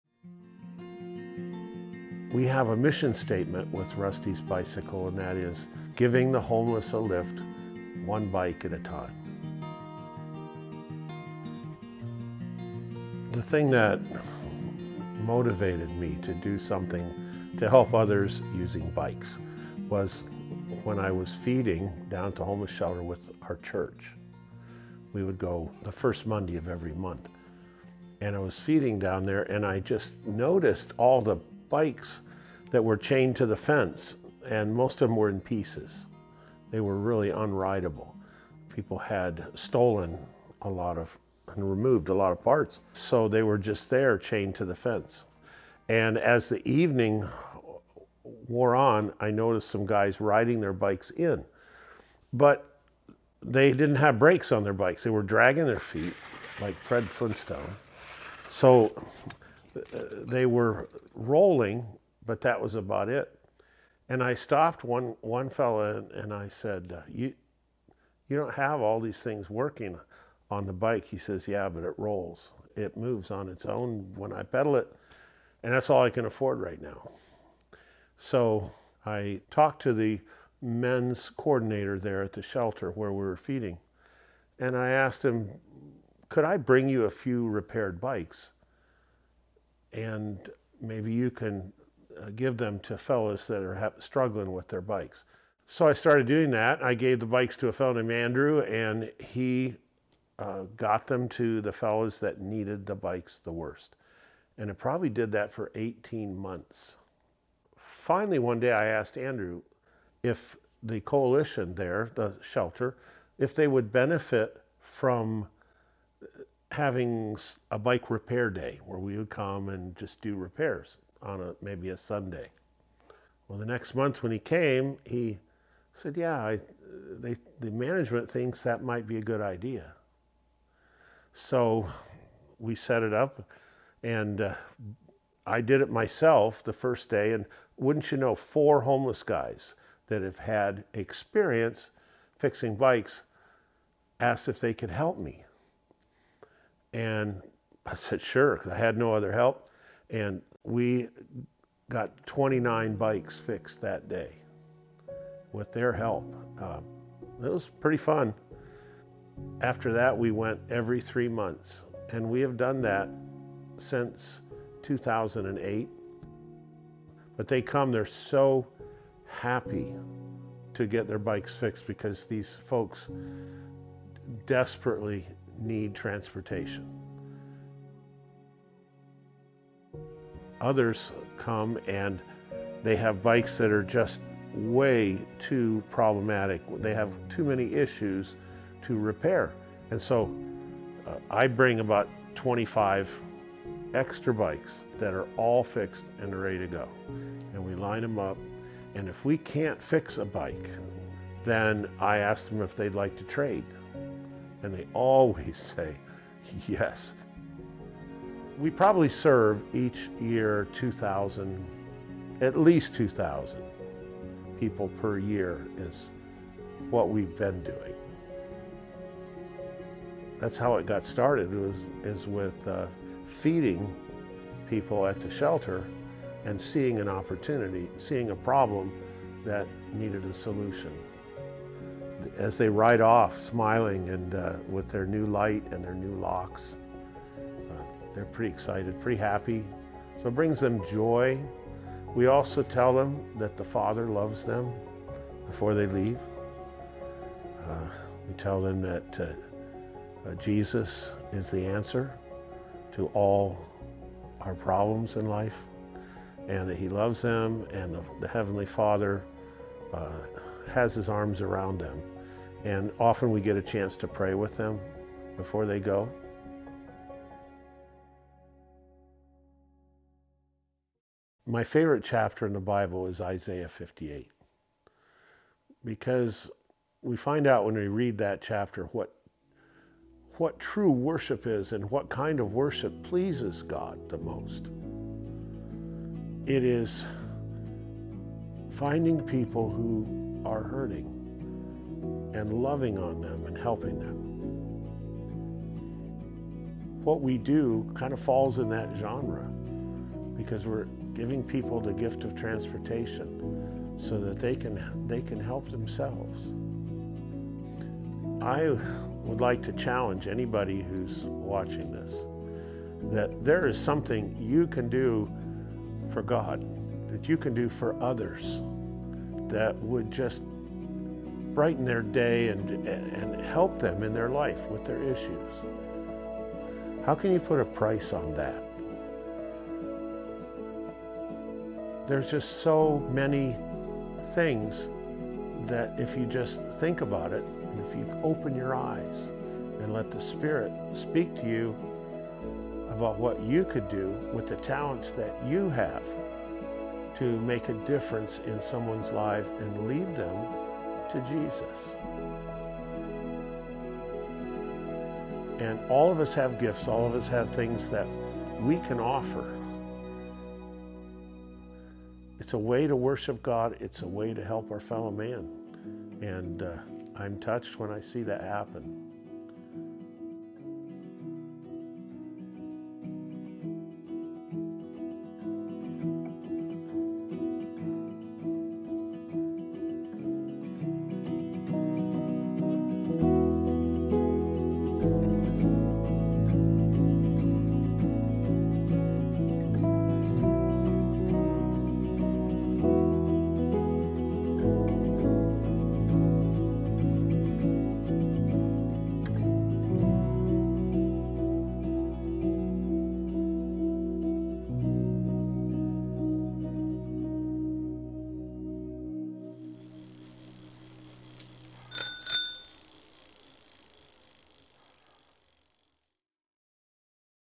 Testimony Now interviews Russ Durham
Giving the homeless a lift, one bike at a time.  That's the heart and mission statement of Russ Durham, founder of Rusty's Bicycle Recycle.
Rusty’s Bicycle Recycle is a 100% volunteer organization, no employees.  We repair donated bicycles and deliver them to partner charities who serve the unhoused, including homeless veterans and foster children.  Many of our volunteers have been associated with the organization since its inception.  Our charity relies on Public Support and donations to repair and deliver over 2000 donated bicycles each year.